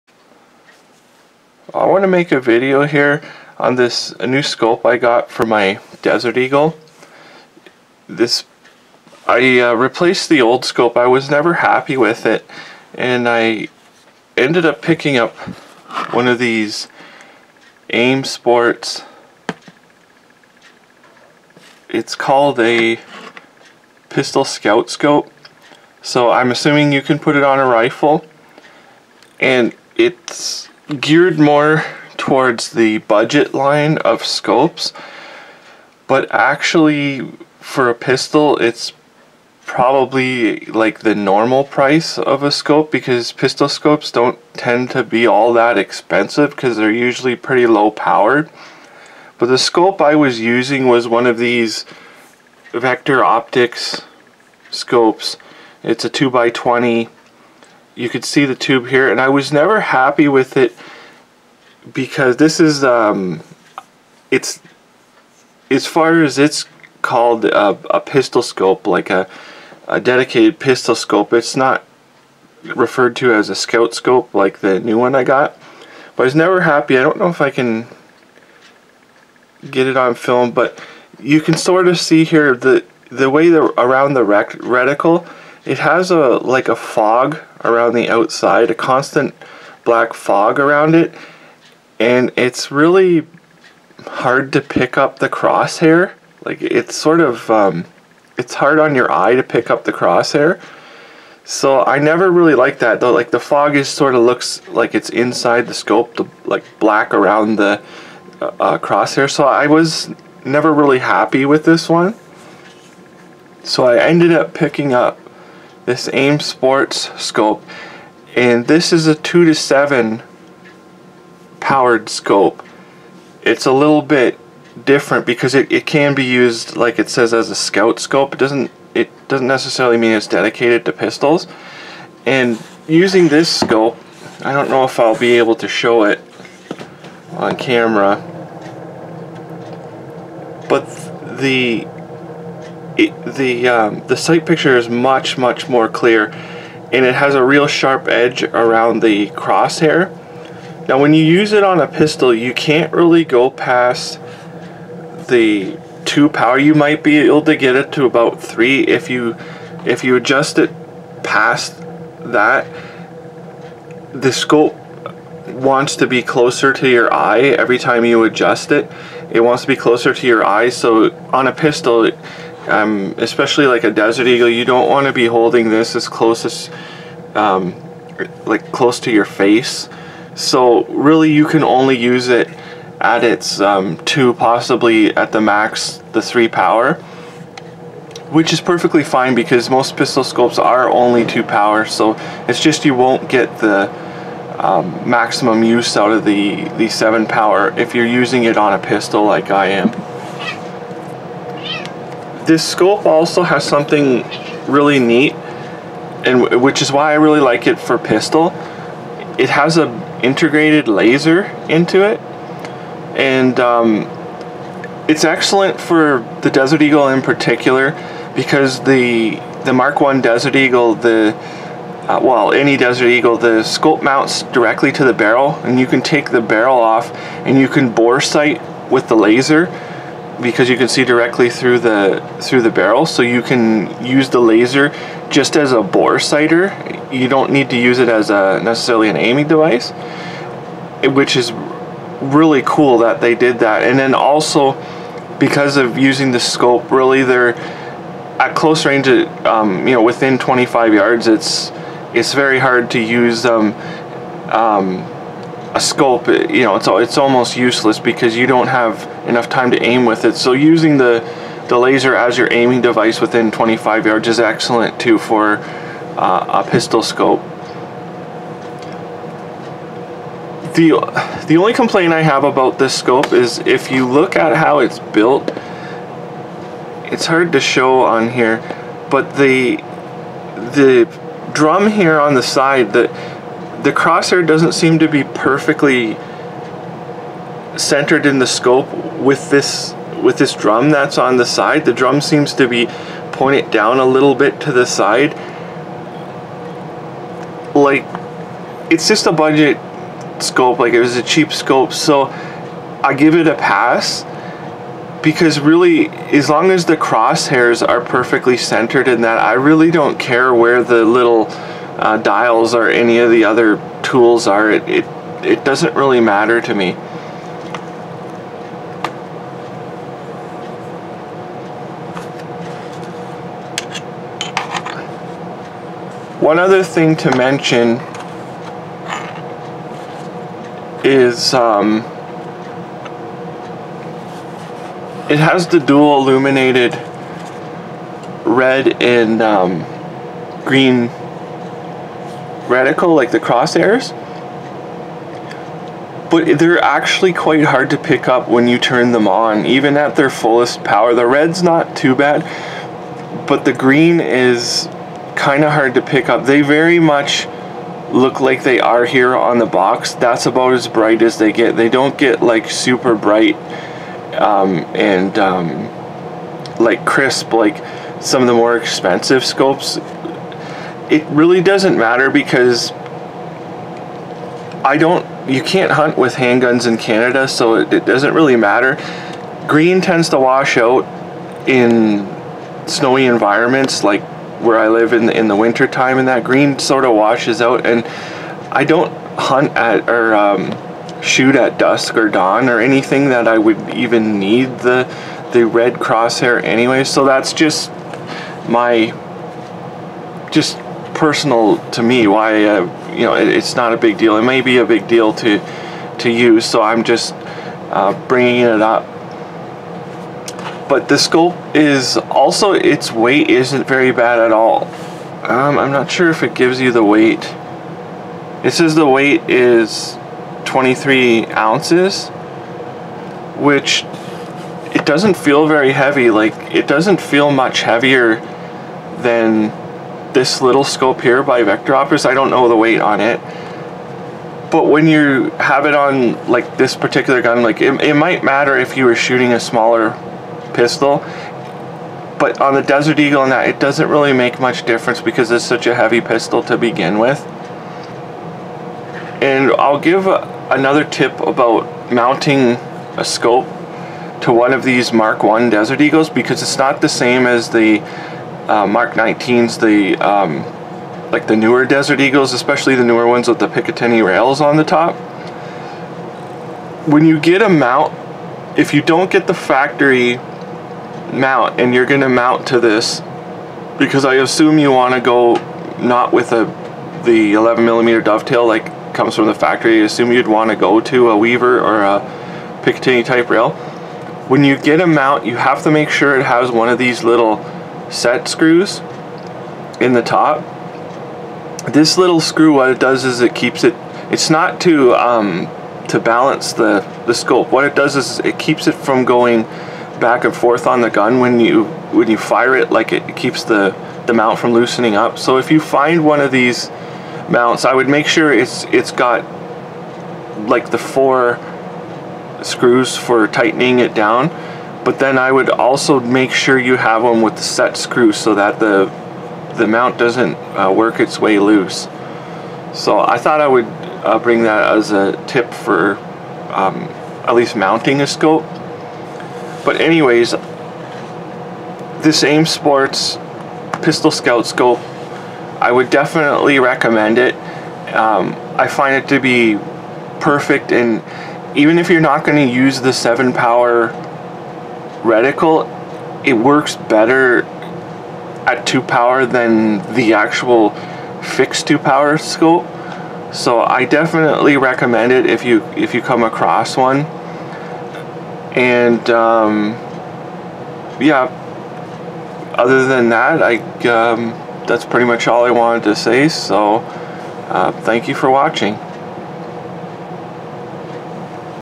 AIM Sports pistol scope on Desert Eagle Mk 1 357 14"
review of the budget AIM sports 2-7X32 Pistol scout scope on desert eagle MK1 357 14" barrel.